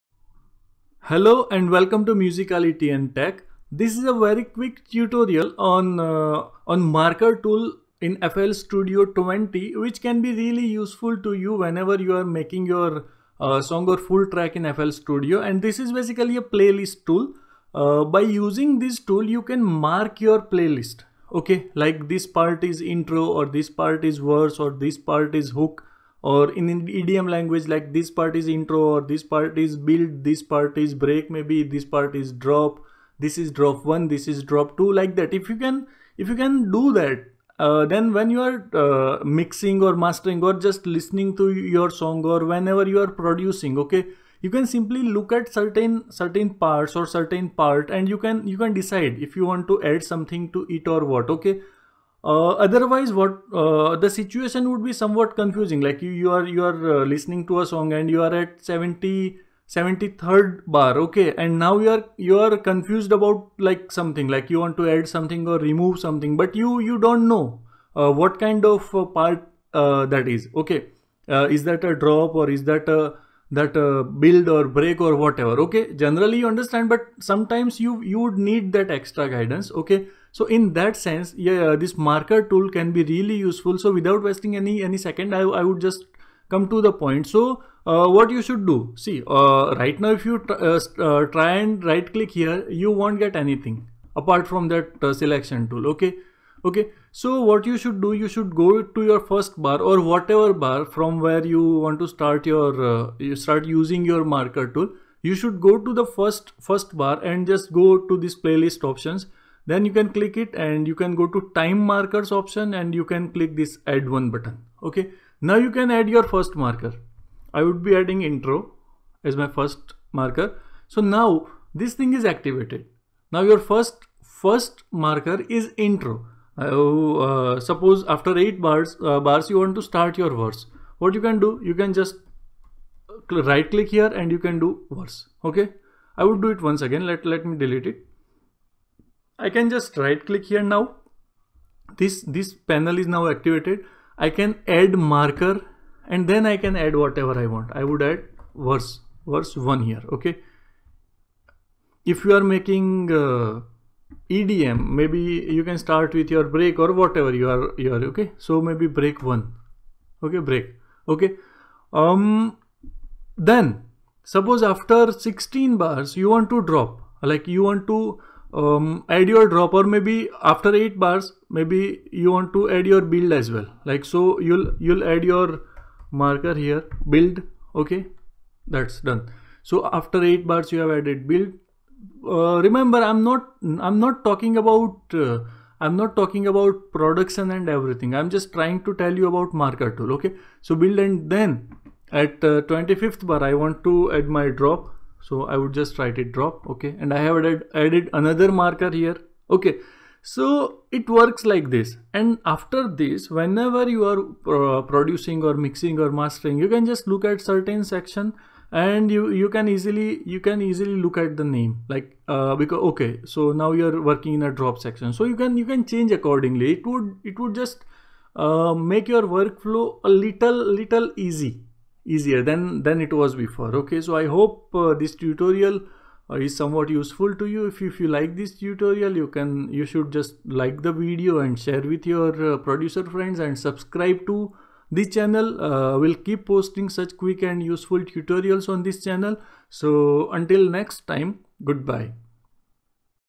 How to add markers in FL Studio 20
Learning how to use the marker tool in FL Studio 20 can greatly improve your music production workflow. In this quick tutorial, I'll try to explain how you can use the marker tool in FL studio 20.